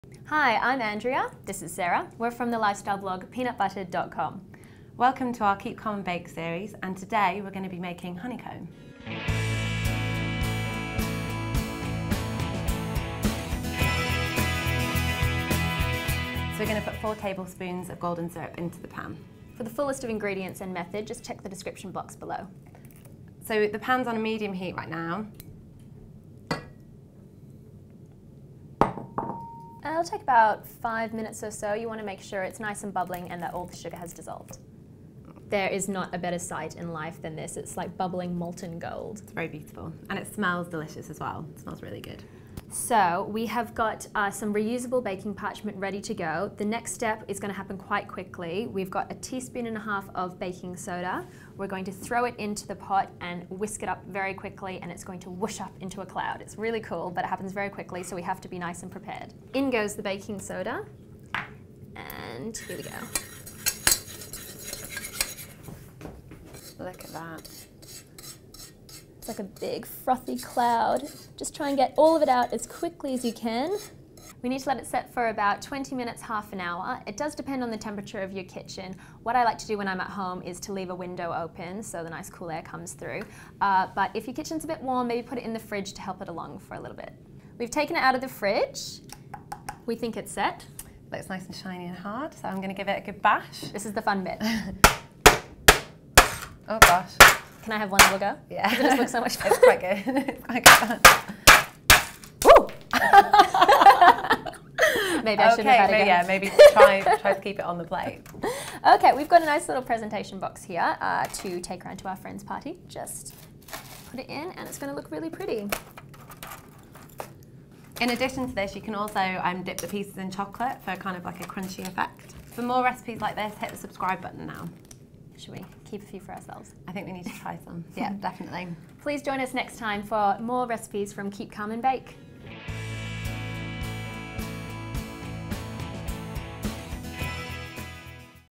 How To Make Honeycomb: Keep Calm And Bake 6
Here's a great treat for a film night in. In this episode of Keep Calm And Bake, Andrea and Sarah show you how to make chewy honeycomb.

Ingredients:

100g caster sugar
4 tbsp golden syrup
1 ½ tsp baking soda

Step 1: Heat
Heat the sugar and golden syrup in a heavy based saucepan until the sugar is dissolved, the liquid is light brown in colour and bubbling. Meanwhile, line a baking tray with well greased foil or a reusable baking parchment.

Step 2: Add Baking Soda
Remove from the heat, add the baking soda and quickly whisk. Pour out onto the baking tray and allow to set in a cool place until hard. Break into pieces.